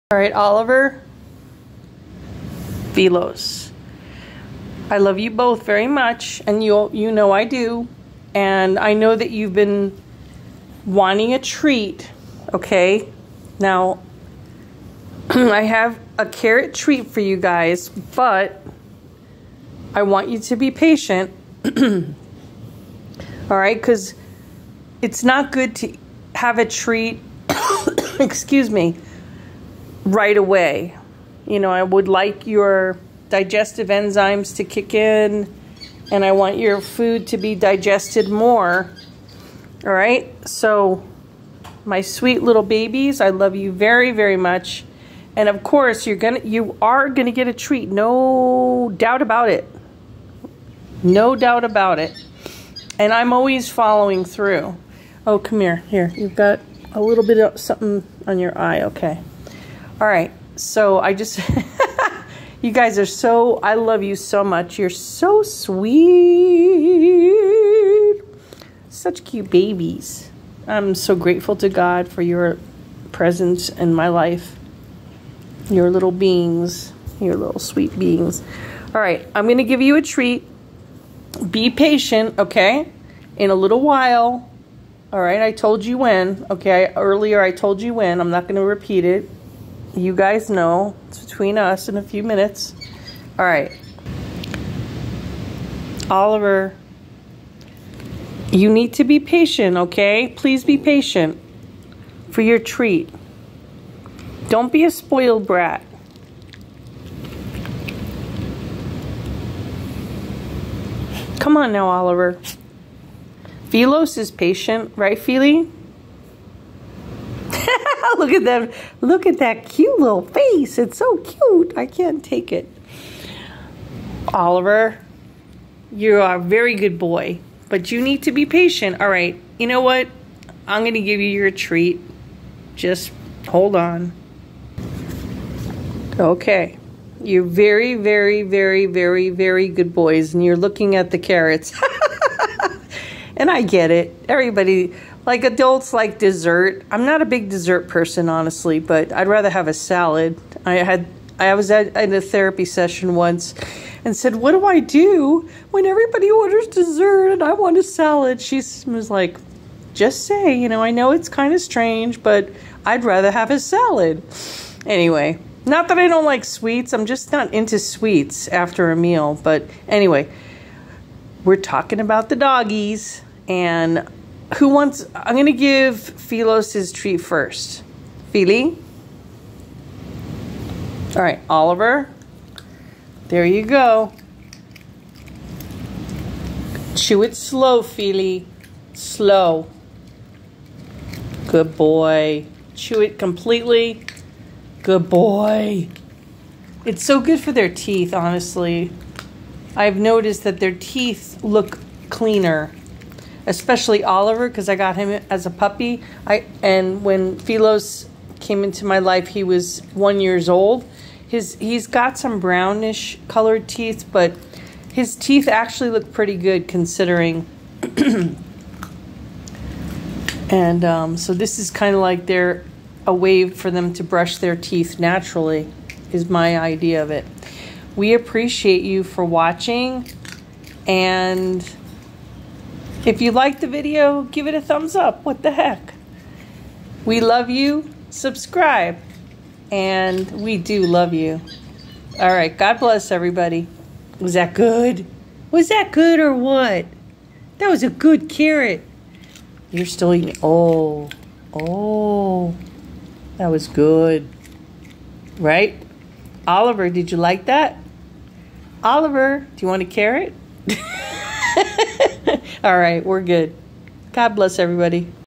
How They Plea For A TREAT!!!!😄🌻💕
I wait a bit, before giving them a treat, after their dinner!

They certainly try to persuade me otherwise!  Sometimes it works🐕🌞😄🌻